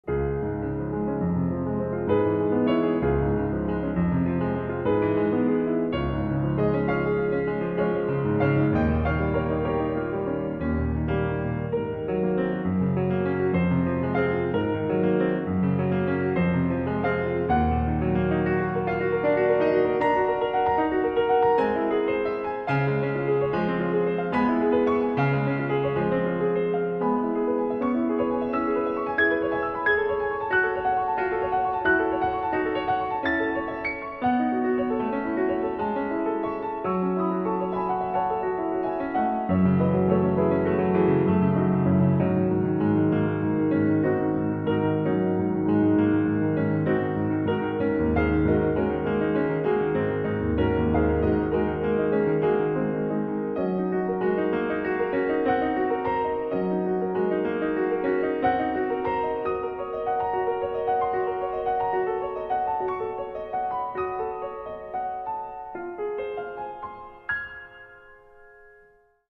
Timelapse | Drawing a school girl | Ingrid Orlando Zon
Traditional pencil & brushes drawing of a school girl | Children's book illustration